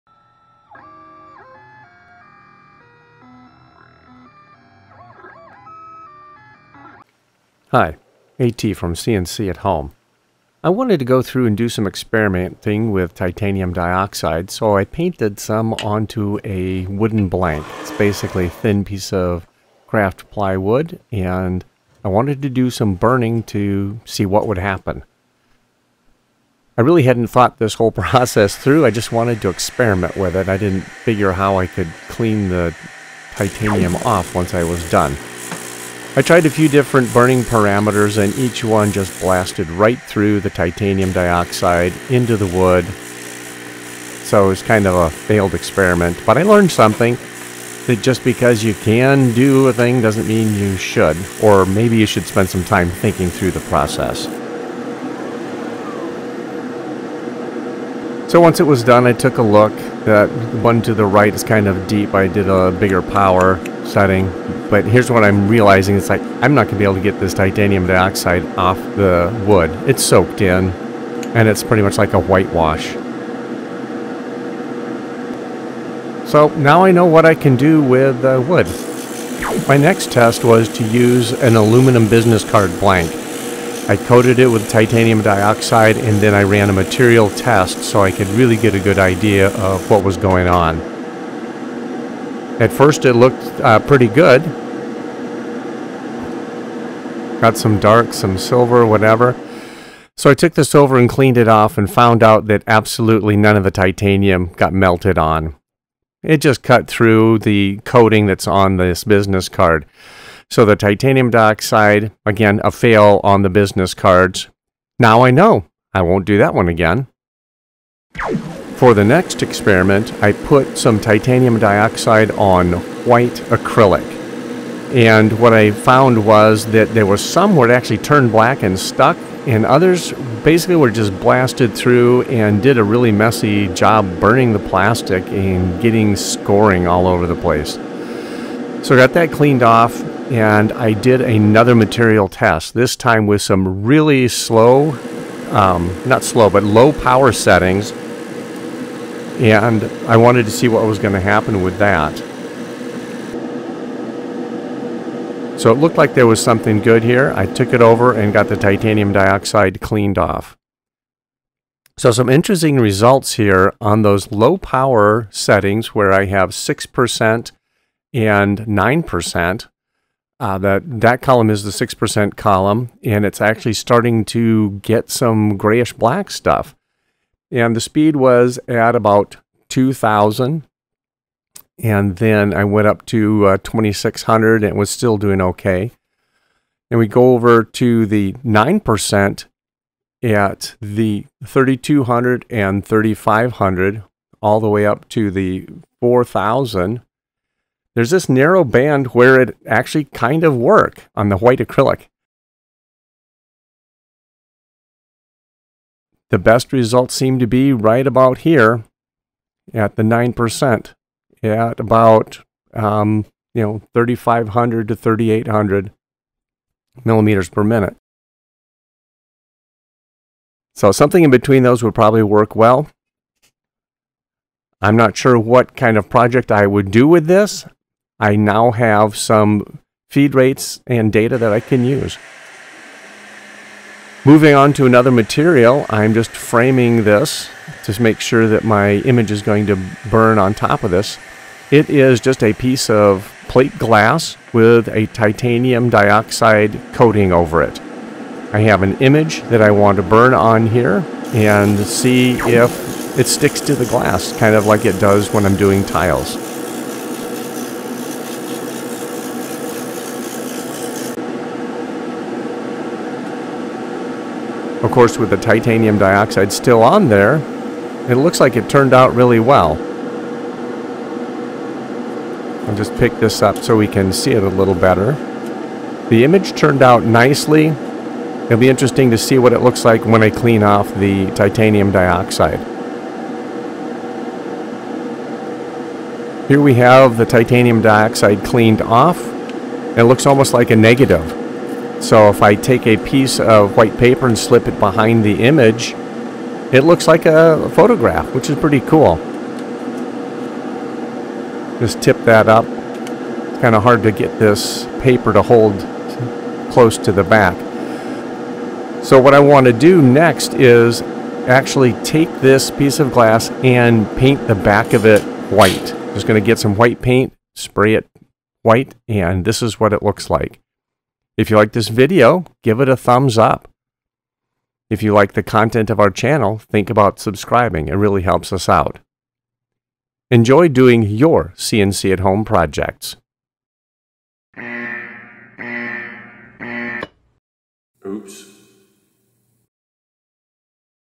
What will Titanium Dioxide (TiO2) stick to other than ceramic tile? Will It TiO2?
I have been working with titanium dioxide for a while now and have gotten my ceramic tile process down.  I asked myself, what else could I use TiO2 on and get similar results?  I tried a few materials and this video shows the results.  Some things worked while others did not.